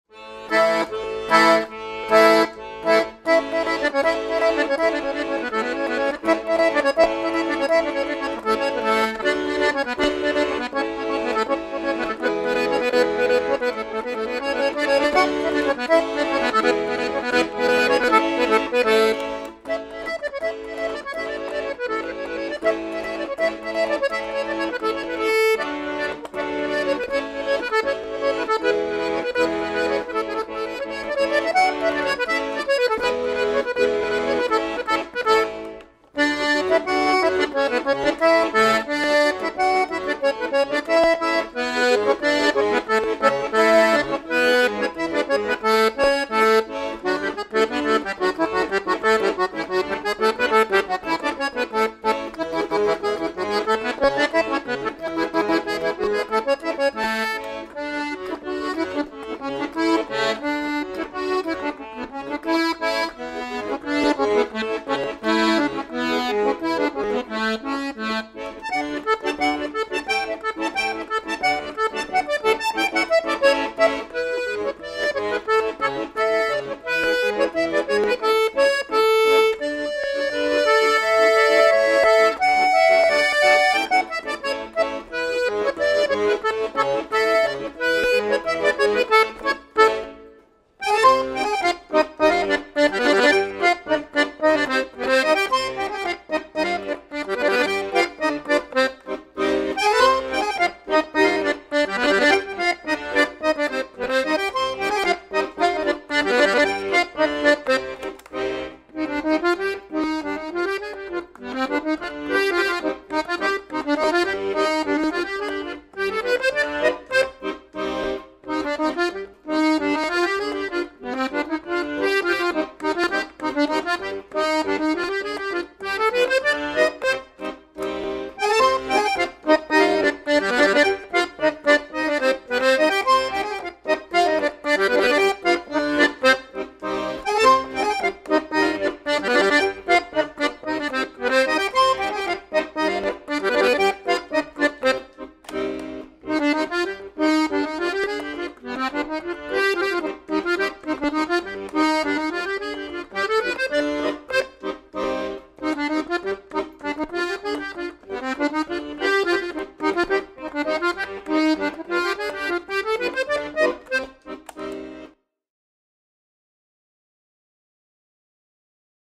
6246 - Ivory Scandalli 120.3 Piano Accordion LMM 41 120 $699
Brand: Scandalli
Model:  120.3
Type: Piano Accordion
Reeds: 3/4 LMM
Treble: 41 Keys, 19.25"(49cm) Key to Key, 1 Registers
Bass: 120 Bass Buttons, 0 Registers
Weight: 19lbs / 8.5kg
Features: Full size accordion with toggle switch between MM and LMM
Includes:  Used Straps and Case, 6 Month Store Warranty